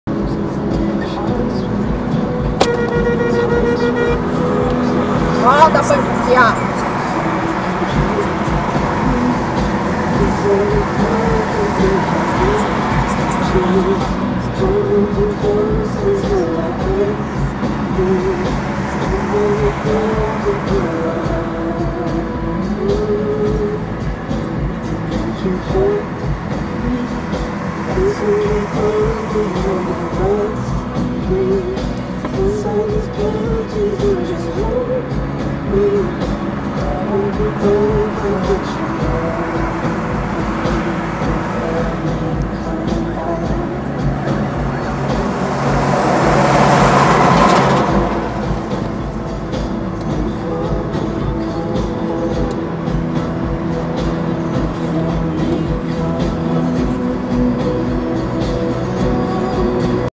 BONAMIX CHEGOU CHEGANDO JÁ MANDOU FAZER PASSARELA AÉREA. FALTA AGORA UM VIADUTO
BONAMIX DE PARABÉNS. SSAPORRA NÃO TEM RETORNO, TU FICA HORAS NA BR DA MORTE, SEM NECESSIDADE. ATÉ PARA IR À PADARIA TEM QUE PEGAR ESSE LIXO.